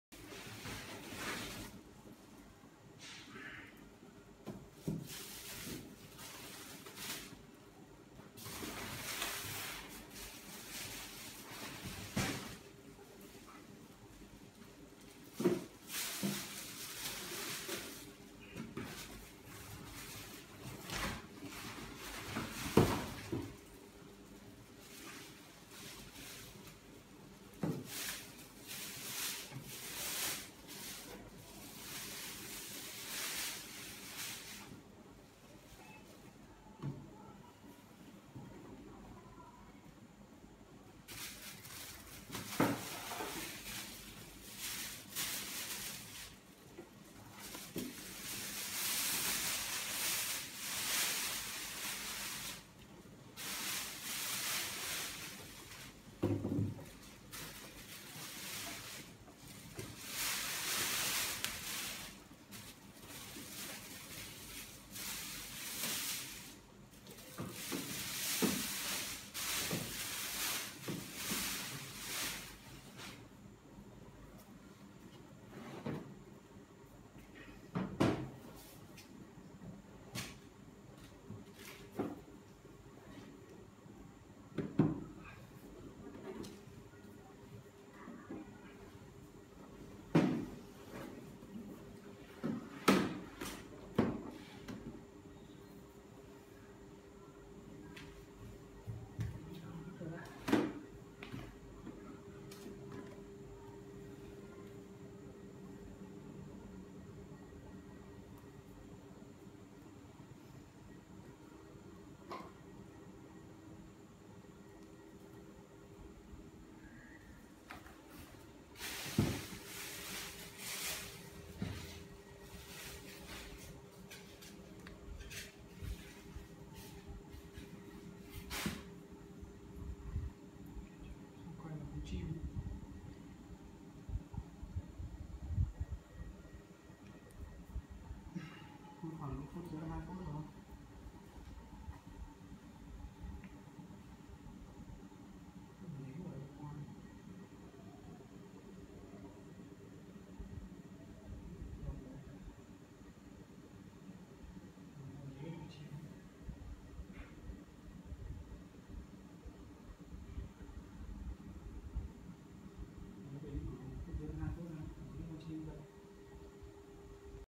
Tranh Treo Tường 3D - Tranh Hoa Mẫu Đơn - 0342 205 997
Tranh Treo Tường 3D - Tranh Hoa Mẫu Đơn -
Hoa mẫu đơn là loài hoa tượng trưng cho sự sang trọng, quý phái, niềm vinh hạnh.
➡️CHUNG CƯ, BIỆT THỰ, VĂN PHÒNG, NHÀ MỚI
🏠 Địa chỉ: Phố Bằng B - Hoàng Liệt - Hoàng Mai - Hà Nội